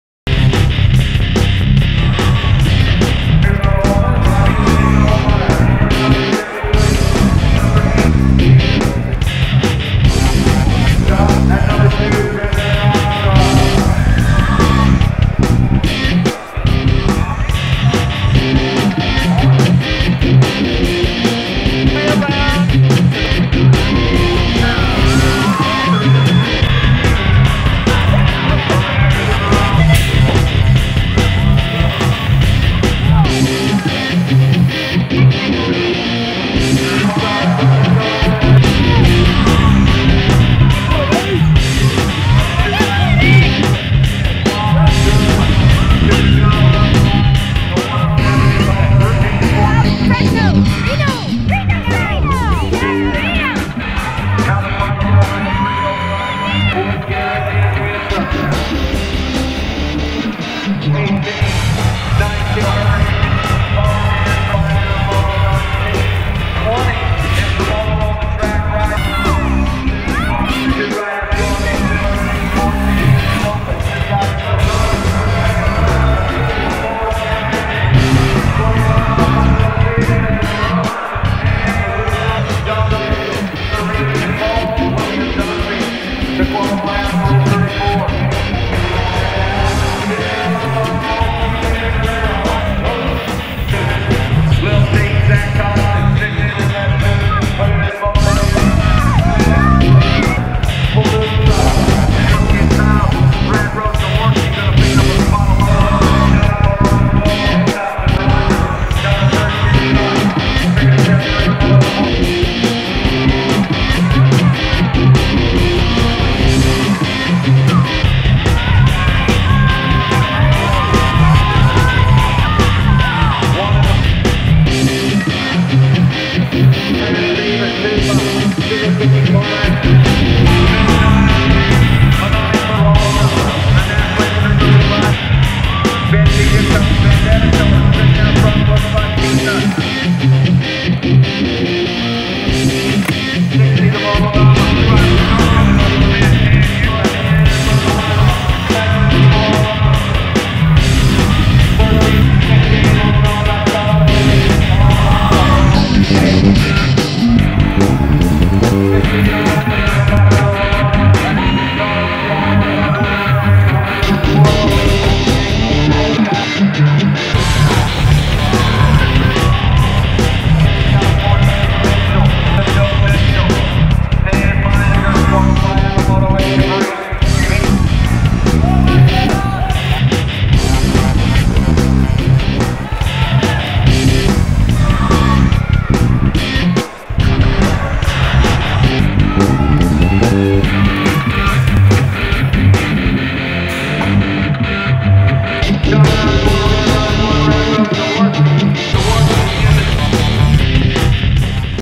ABA BMX Nationals 2012: Reno, NV - Kids
are on location in Reno, Nevada for the annual Silver Dollar Nationals! We love this event. Such anticipation, competition, family style BMX'ing. The moto heats were hot!